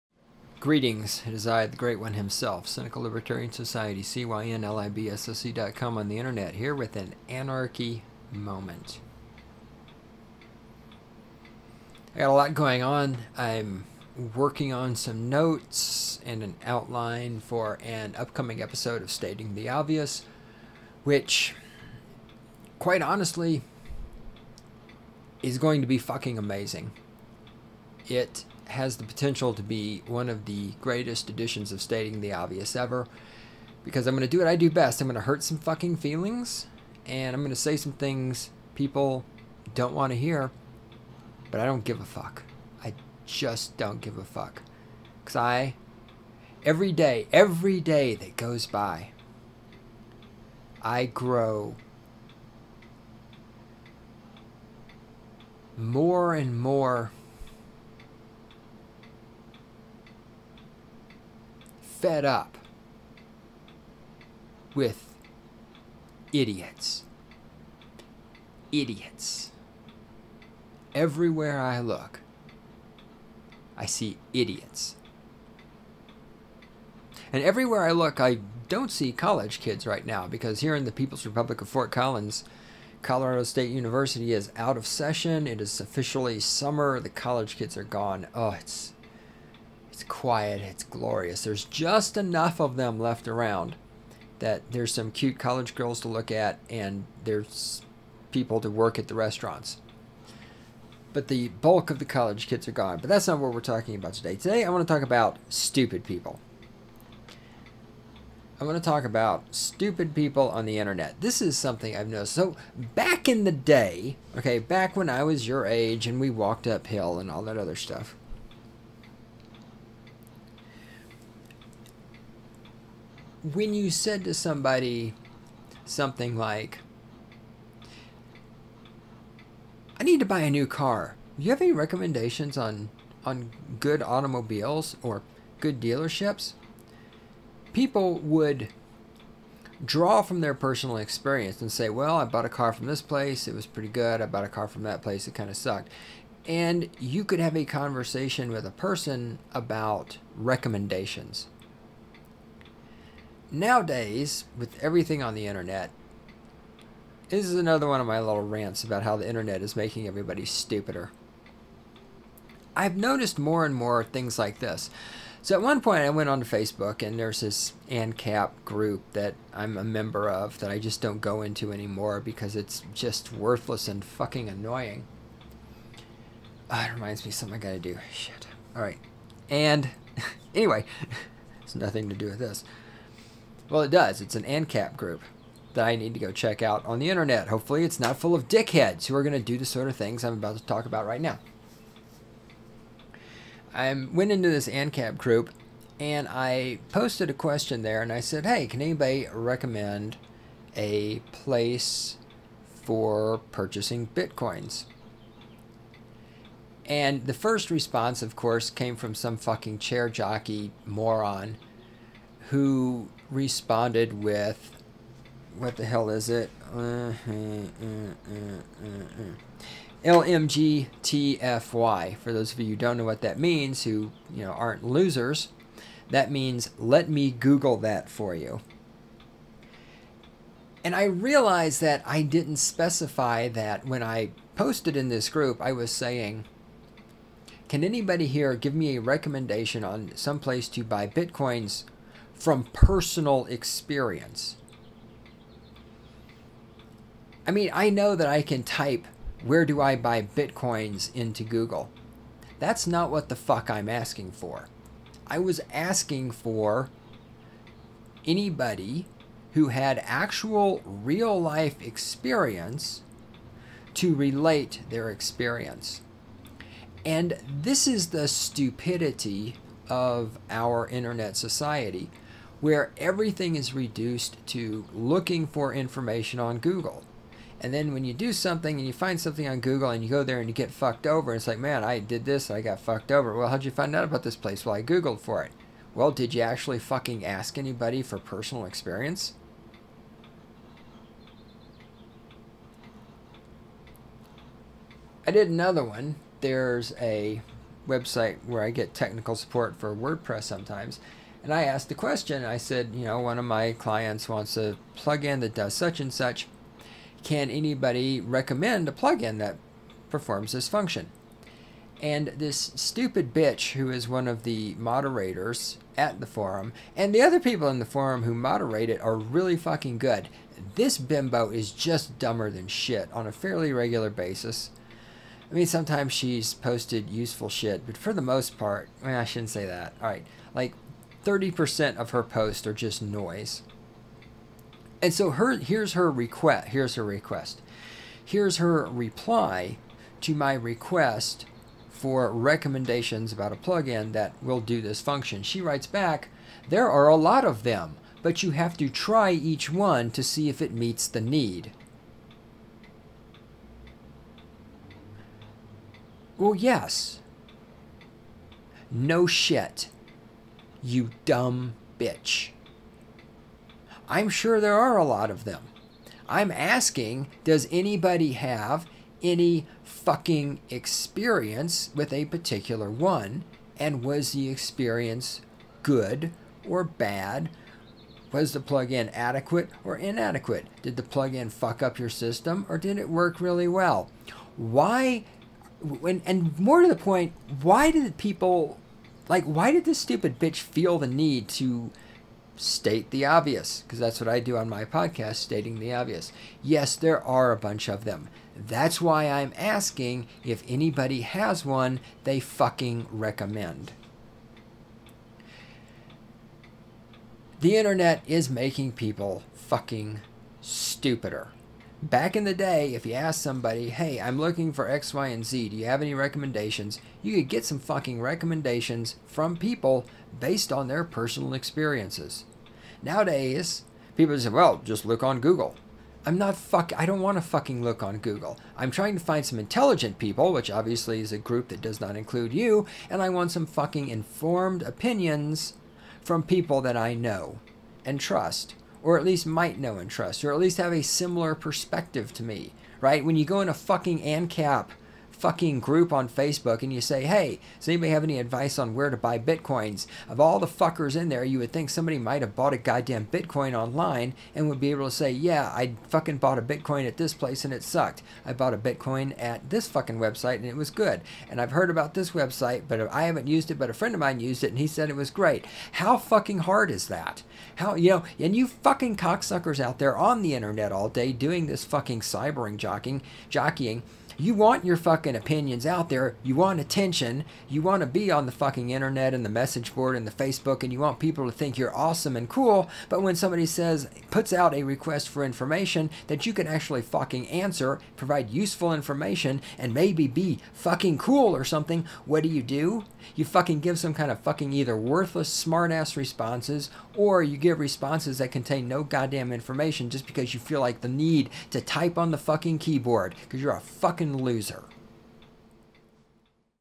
Anarchy Moment #0065 -- I Grow Tired of Idiots on the Interwebz.
Did you notice that stupid people use the internet as well? Yup. They are all over the place. Being able to easily communicate with other people around the world seemed like a great idea. Until it sinks in that most of the people on the internet for more than 10 minutes a day are idiots